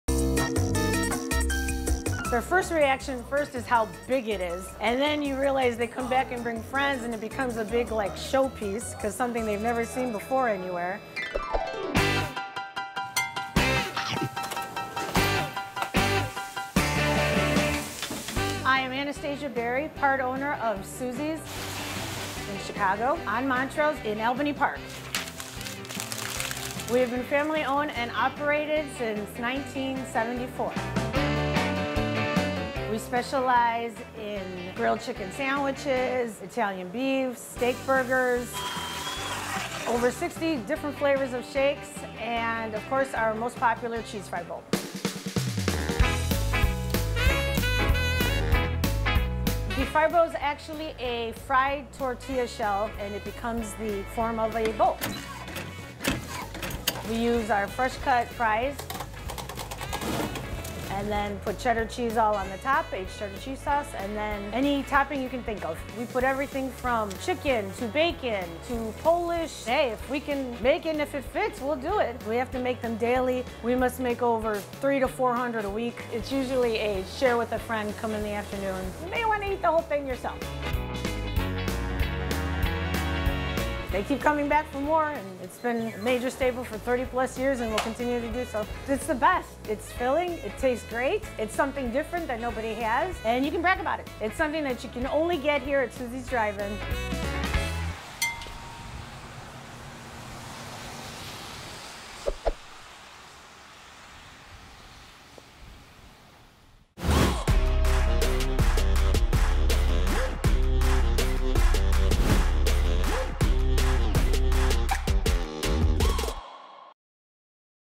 Cheese Fry Bowl at Susie's Drive Thru | Bite Size
Susie's Drive Thru is a Chicago institution. This family-owned and operated restaurant is known for its outrageous French Fry Bowls. They make between 300 and 400 a week! It starts with a taco salad bowl made from scratch. Then fill it with French fries, smother it in cheese, and add any meat of your choosing. DELISH!